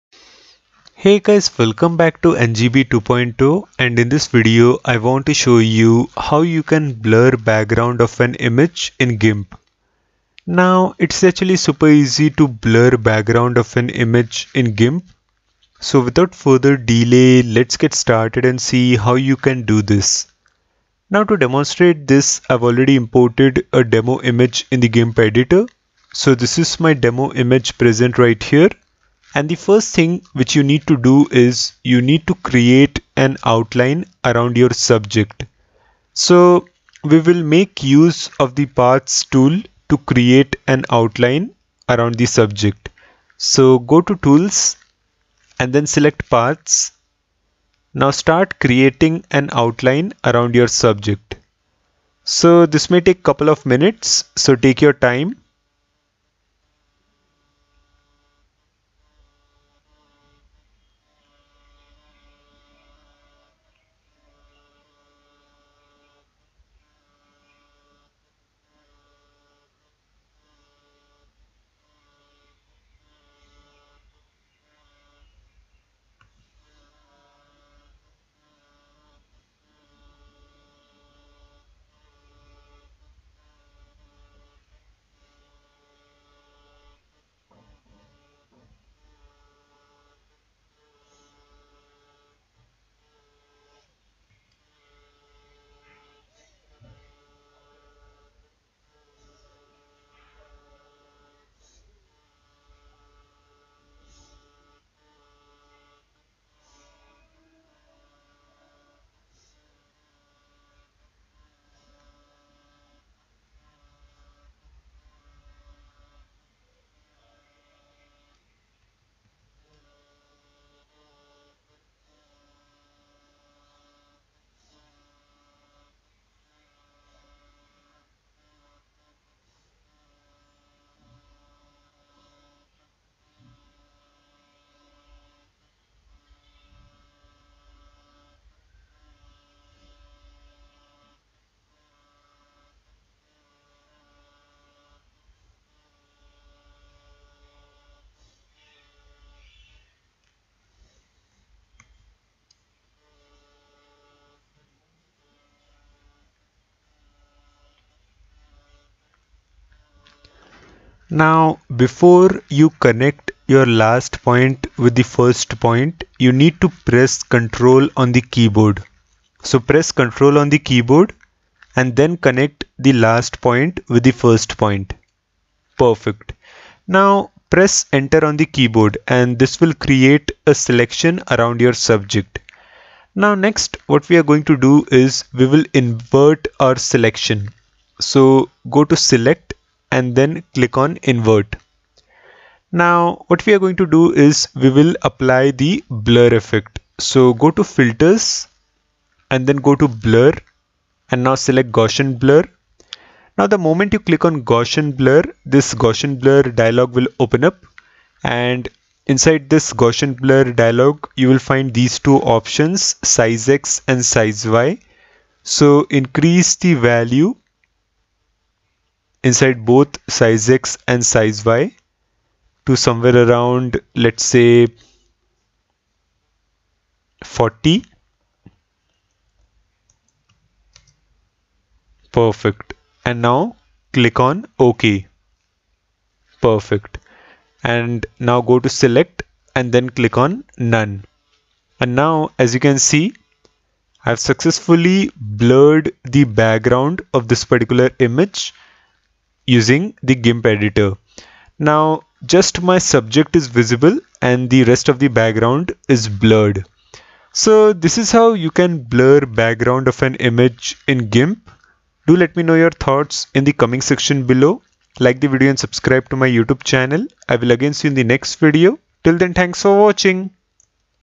How to blur background using GIMP
In this tutorial,I show you how you can blur background of a photo in GIMP.